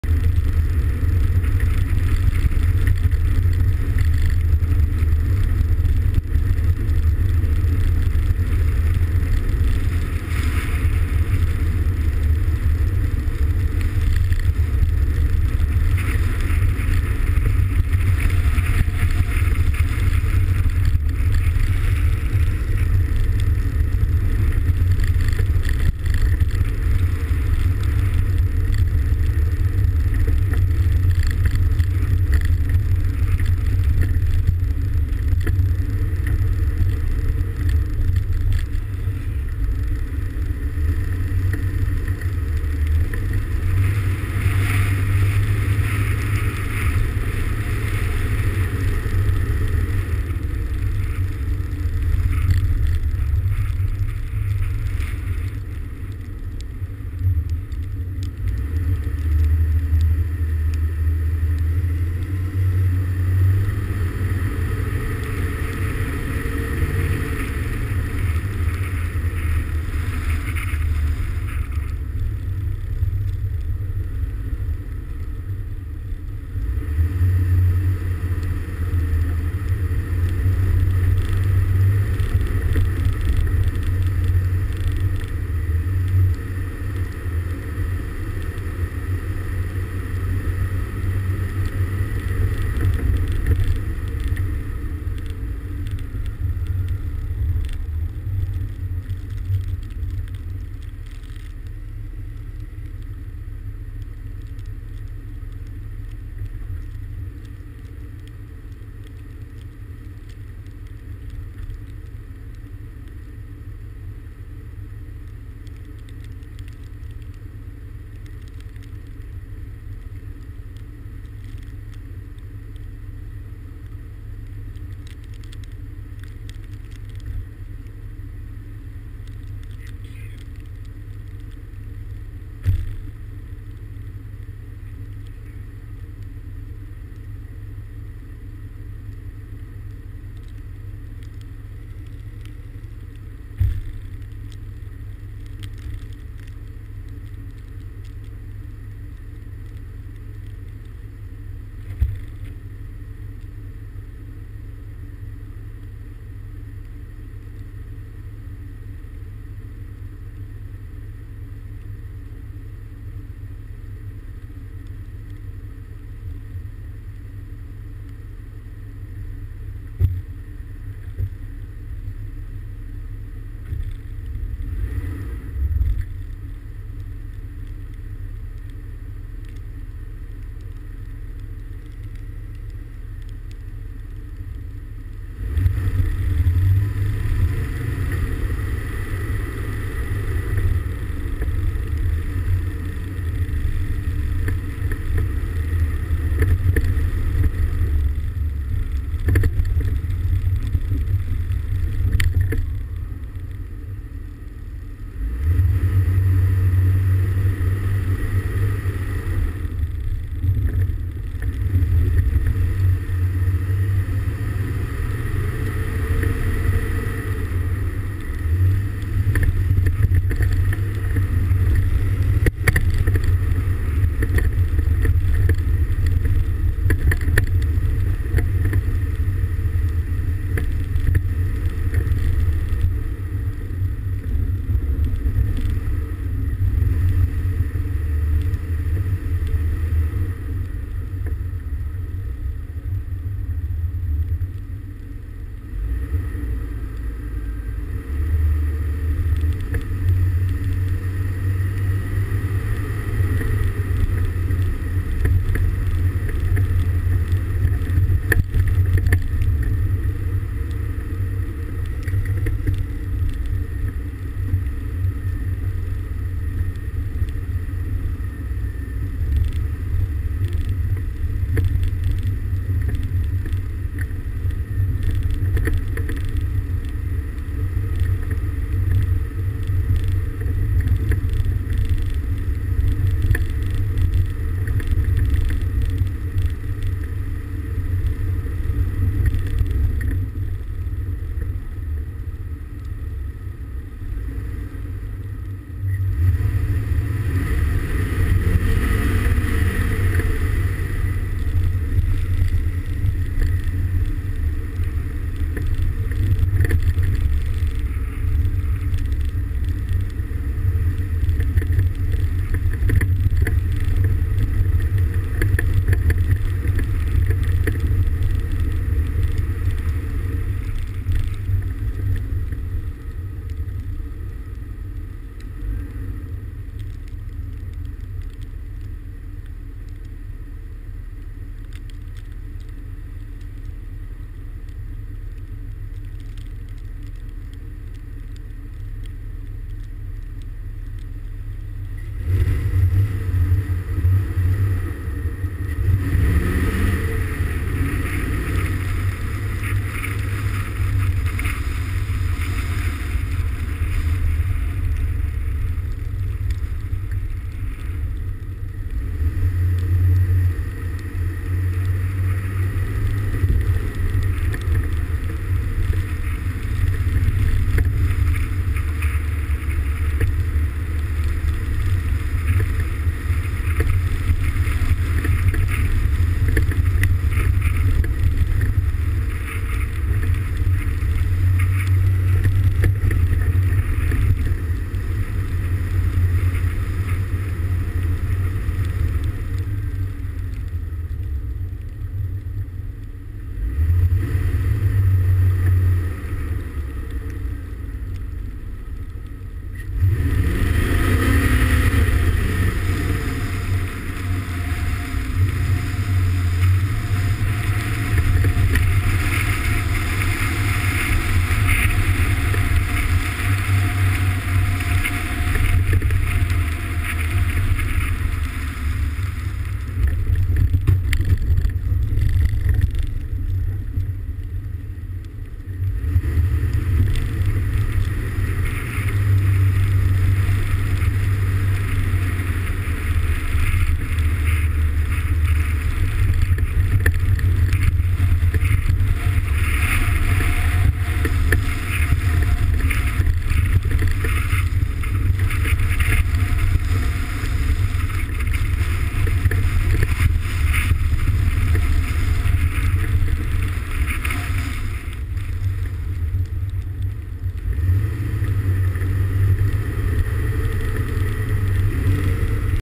Tulsa Police on Snow Patrol 02/09/2011
Our Cadillac Escalade that was confiscated from a drug dealer was on patrol in the area of the University of Tulsa. As you can see the main roads are drivable and the side streets were still dangerous.